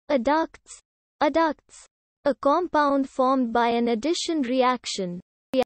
'Adducts' Meaning and Pronunciation
Word: Adducts
Meaning: a compound formed by an addition reaction